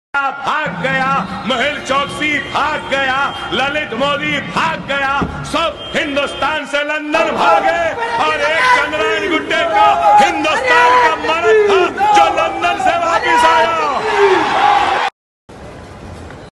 Akbaruddin Owaisi Sahab At Rajiv Gandhi International Airport Back to Hyderabad today! WhatsApp stat
AIMIM Party Floor Leader Habeeb E Millat Akbaruddin Owaisi Sahab At Rajiv Gandhi International Airport Back to Hyderabad today!